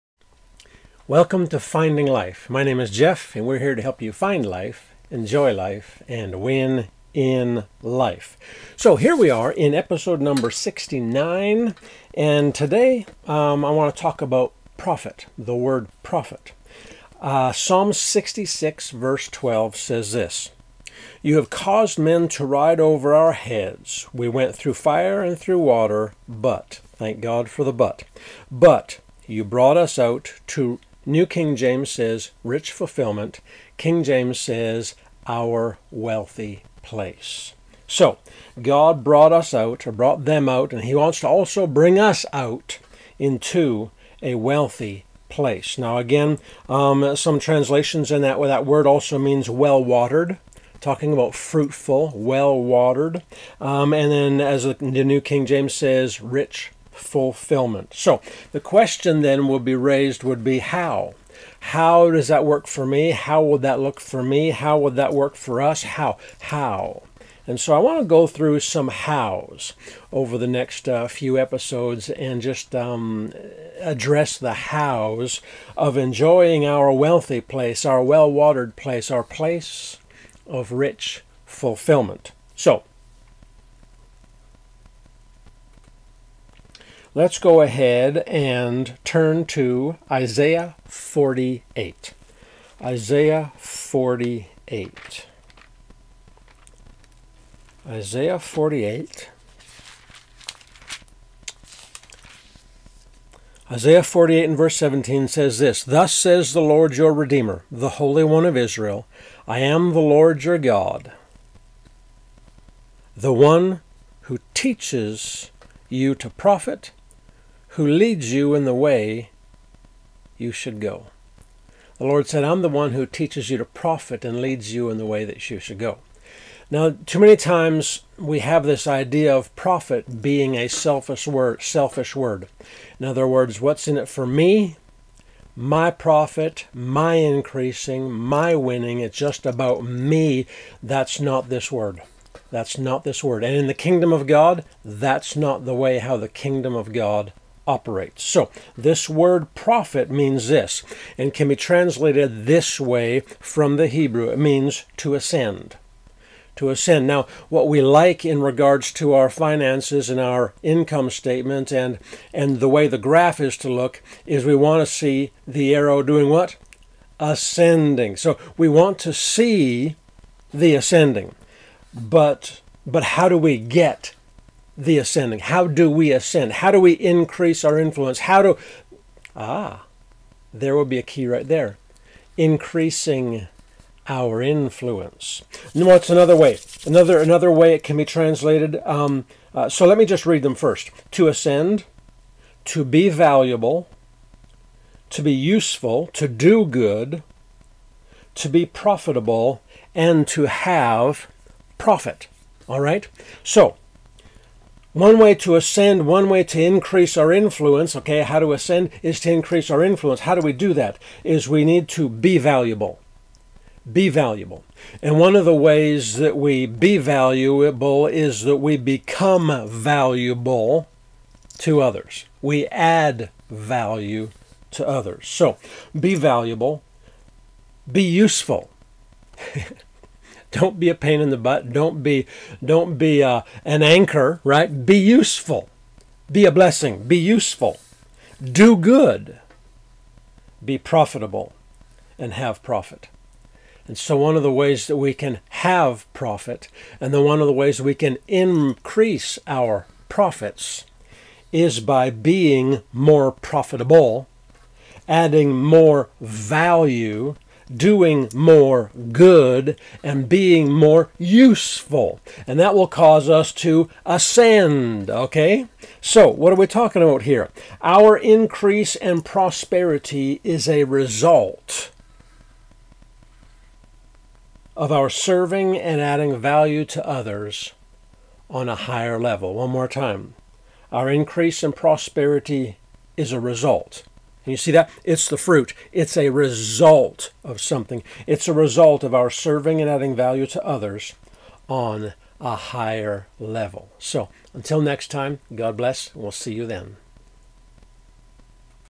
FL 069 Profit
Episode 69
Profit

Ps 66:12 tells us that God brought them out to a wealthy place.

What is a wealthy place?

How can we enjoy our wealthy place, our place of rich fulfillment?

Let's begin to go through some "how's" and find out.

What does it mean to profit? - and it is not what most people think.

Find out more in today's training...